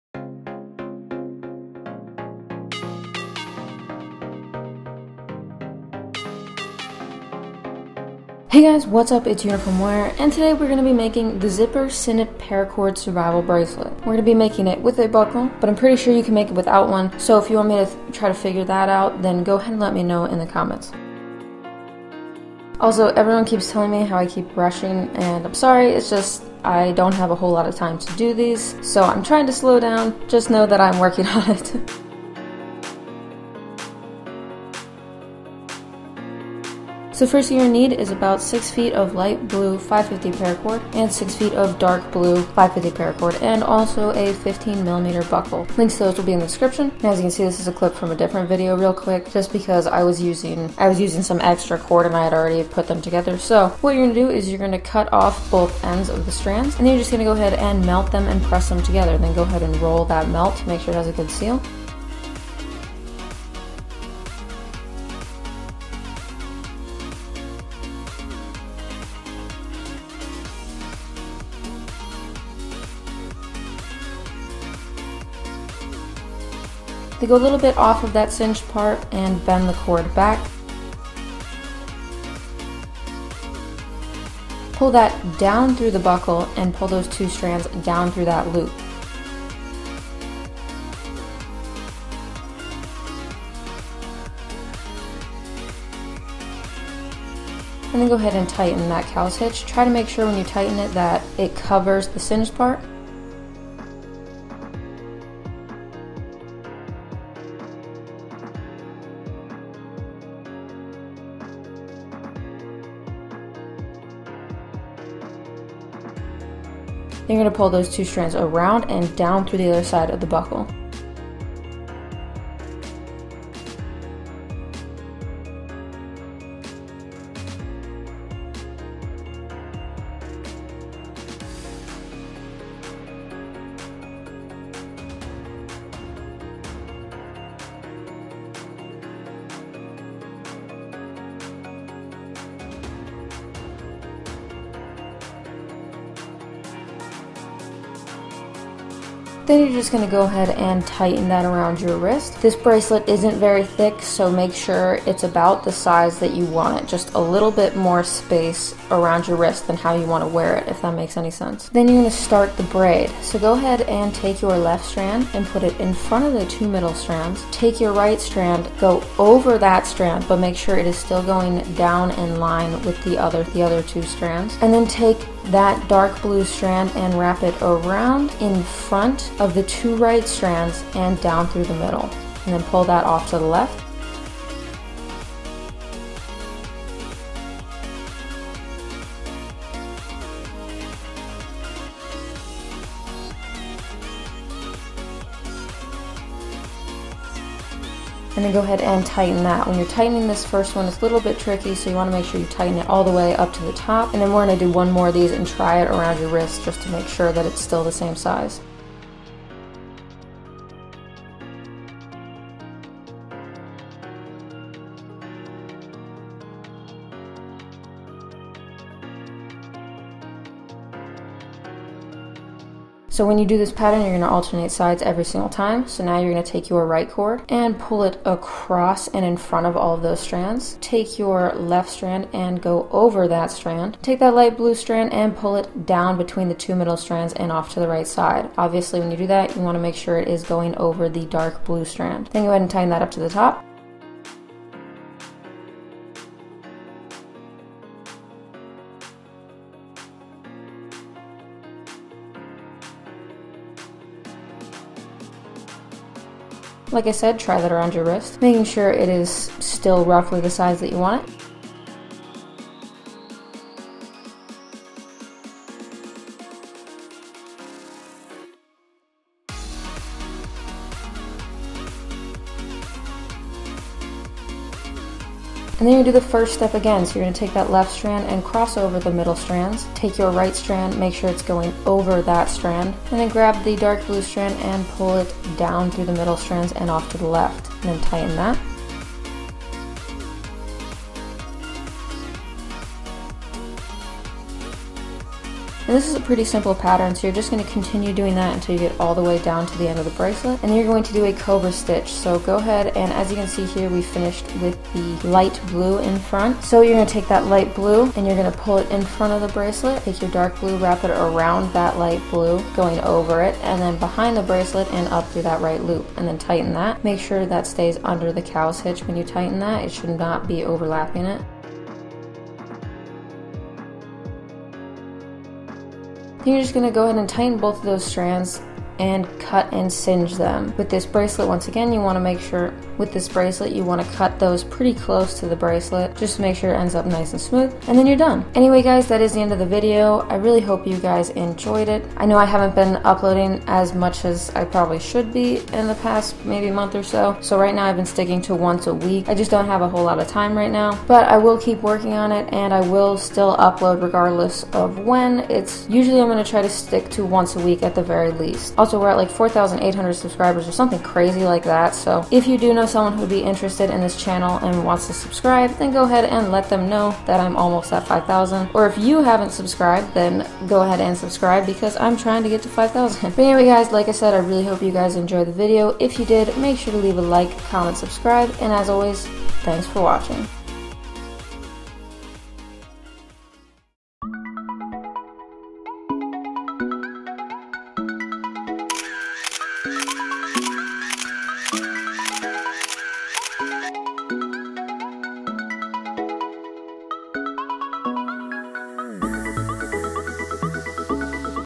How to make The Zipper Sinnet Paracord Survival Bracelet [With Buckle] [Tutorial]
Today's video shows you how to make an easy bracelet with a buckle that only takes about 15 minutes :) If you have any questions, leave them in the comments ;)

Original video by BoredParacord (I forgot to mention it in the video but this is where I first saw this pattern.)

Send me a picture of your paracord bracelet!

Paracord Used in This Video
All the measurements are custom to me. I have about a 6½in wrist.